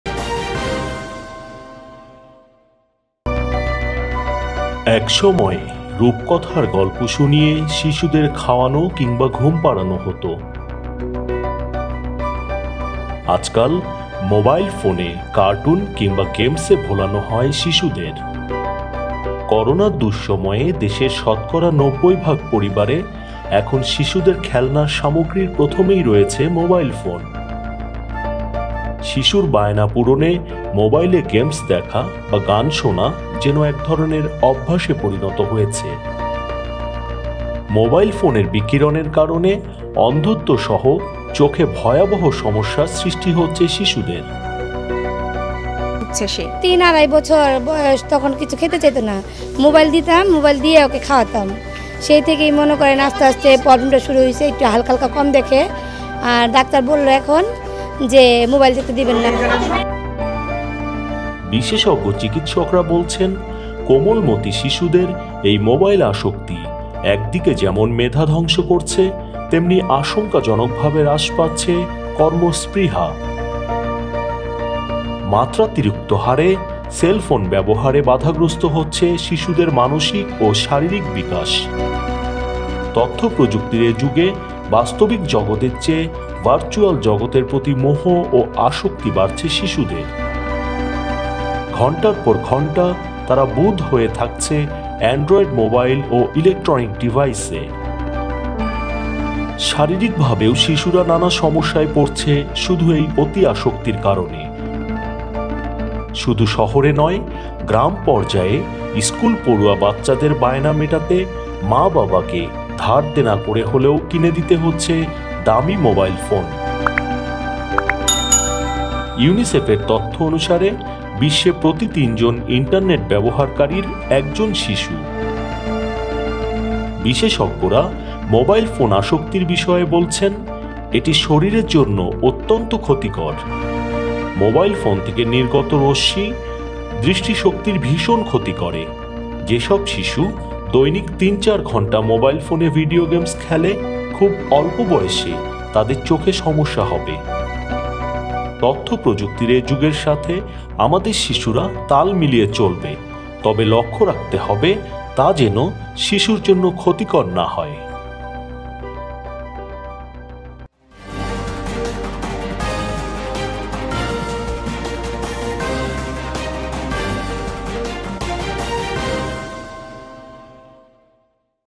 মোবাইল এর ক্ষতিকর রশ্মি আপনার শিশুর ব্যাপারে আপনি কি সচেতন
এক সময় রূপকথার গল্প শুনিয়ে শিশুদের খাওয়ানো কিংবা ঘুম পাড়ানো হতো। আজকাল মোবাইলে কার্টুন কিংবা গেমসে ভোলানো হয় শিশুদের।

করোনার দুঃসময়ে দেশের শতকরা ৯০ ভাগ পরিবারে এখন শিশুদের খেলনার সামগ্রীর প্রথমেই রয়েছে মোবাইল ফোন। শিশুর বায়না পূরণে মোবাইলে গেমস দেখা বা গান শোনা যেন এক ধরনের অভ্যাসে পরিণত হয়েছে।
মোবাইল ফোনের বিকিরণের কারণে অন্ধত্বসহ চোখে ভয়াবহ সমস্যা সৃষ্টি হচ্ছে শিশুদের।

বিশেষজ্ঞ চিকিৎসকরা বলছেন, কোমলমতি শিশুদের এই মোবাইল আসক্তি একদিকে যেমন মেধা ধ্বংস করছে, তেমনি আশঙ্কাজনকভাবে হ্রাস পাচ্ছে কর্মস্পৃহা। মাত্রাতিরিক্ত হারে সেলফোন ব্যবহারে বাধাগ্রস্ত হচ্ছে শিশুদের মানসিক ও শারীরিক বিকাশ। তথ্য প্রযুক্তির এ যুগে বাস্তবিক জগতের চেয়ে ভার্চুয়াল জগতের প্রতি মোহ ও আসক্তি বাড়ছে শিশুদের। ঘন্টার পর ঘন্টা তারা বুঁদ হয়ে থাকছে অ্যান্ড্রয়েড মোবাইল ও ইলেকট্রনিক ডিভাইসে। শারীরিকভাবেও শিশুরা নানা সমস্যায় পড়ছে শুধু এই অতি-আসক্তির কারণে। শুধু শহরে নয়, গ্রামপর্যায়ে স্কুলপড়ুয়া বাচ্চাদের বায়না মেটাতে মা বাবাকে ধারদেনা করে হলেও কিনে দিতে হচ্ছে দামি মোবাইল সেট।

ইউনিসেফের তথ্য অনুসারে, বিশ্বে প্রতি তিনজন ইন্টারনেট ব্যবহারকারীর একজন শিশু।

বিশেষজ্ঞরা মোবাইল ফোন আসক্তির বিষয়ে বলেছেন, এটি শরীরের জন্য অত্যন্ত ক্ষতিকর। মোবাইল ফোন থেকে নির্গত রশ্মি দৃষ্টিশক্তির ভীষণ ক্ষতি করে। যেসব শিশু দৈনিক তিন - চার ঘণ্টা মোবাইল ফোনে ভিডিও গেমস খেলে, খুব অল্পবয়সে তাদের চোখের সমস্যা হবে।

তথ্য প্রযুক্তির এ যুগের সাথে আমাদের শিশুরা তাল মিলিয়ে কবে তবে লক্ষ্য রাখতে হবে তা যেন শিশুদের জন্য ক্ষতিকর না হয়।